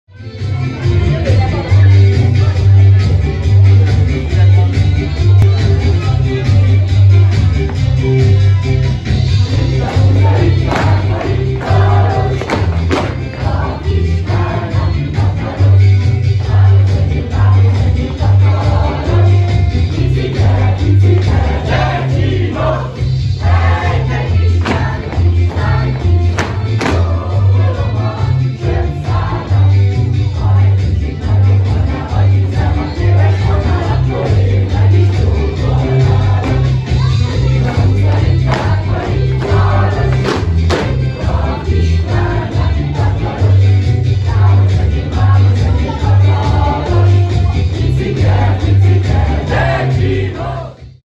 220917 Kisapostag Szüreti mulatság
A tánc főpróbán jártam, ahol műkedvelő fiatalok adják elő a táncukat.